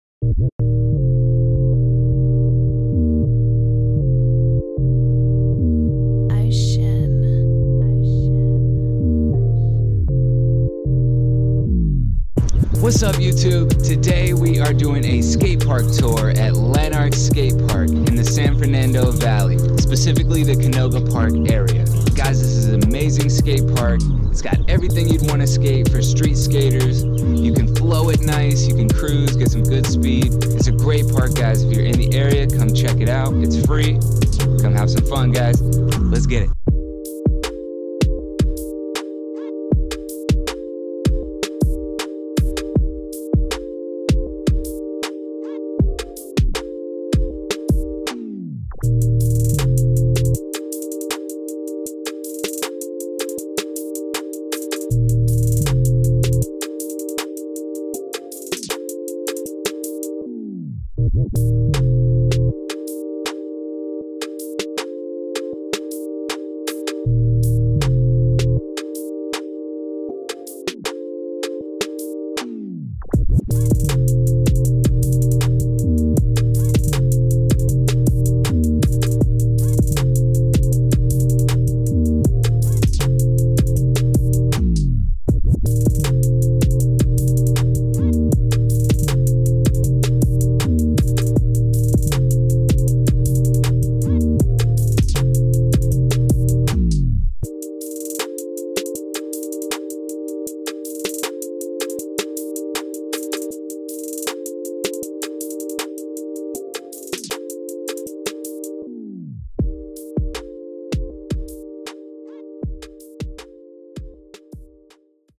Lanark Skatepark | Skatepark Tours
WHYELFILES Presents Skate Tours! Where we showcase Skateparks around the Globe.

Today we visit Lanark Skatepark.
Located in Canoga Park, CA.